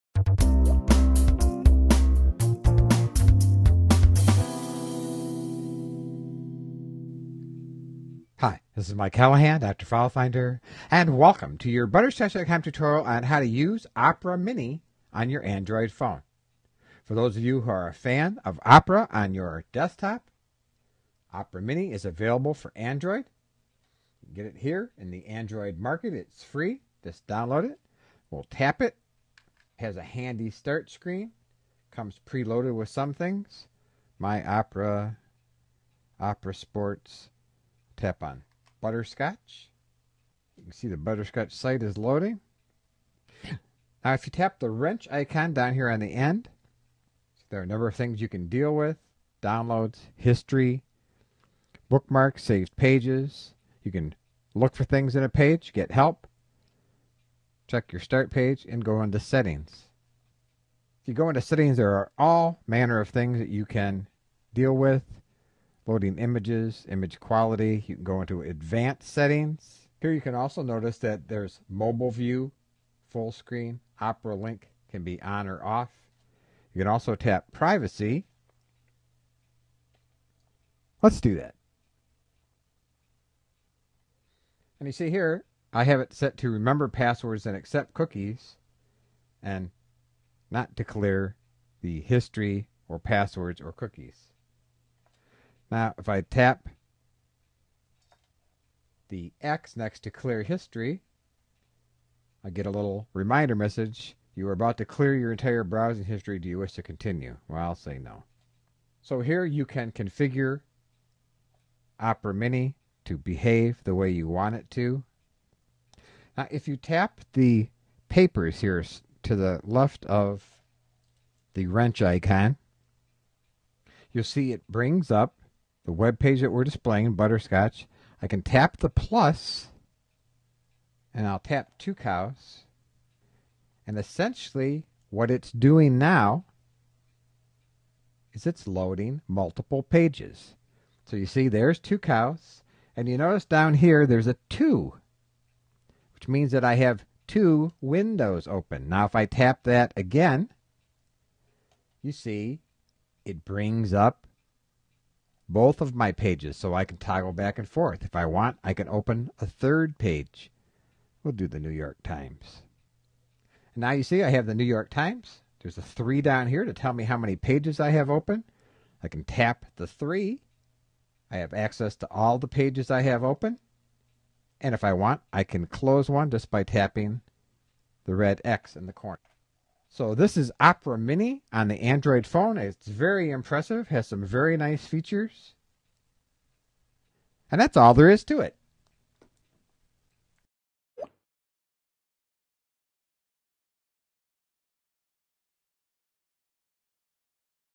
How to use Opera Mini on your Android phone
If you're an Android user who's looking for a full-featured browser then look no farther than Opera Mini. It's a fast and free browser that offers some great features like tabbed browsing, bookmarks and more @ butterscotch.com